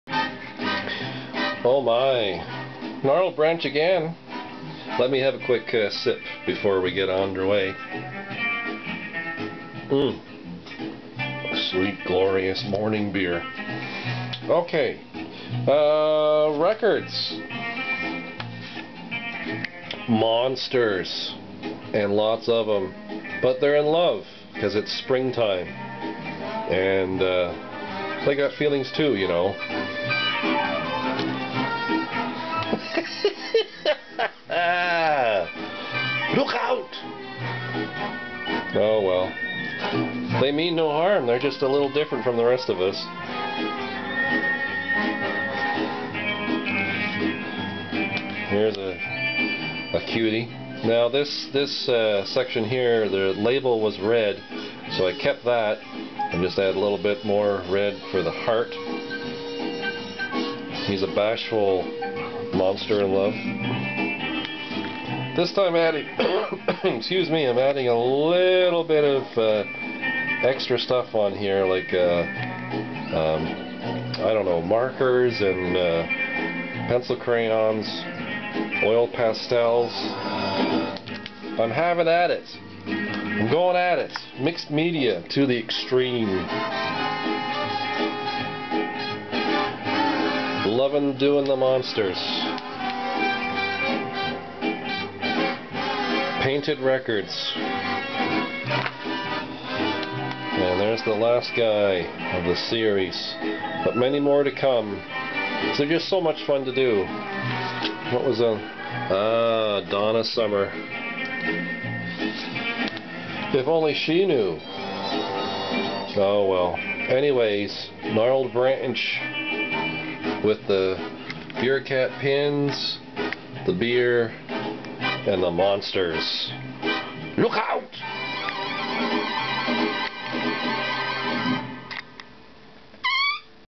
Monster Record Art
recycling, reusing, repurposing damaged records that were destined for landfill are now paintings that just require a pushpin to mount on the wall.

For my artwork printed on products (Prints, Clothing, Home Décor and more) check out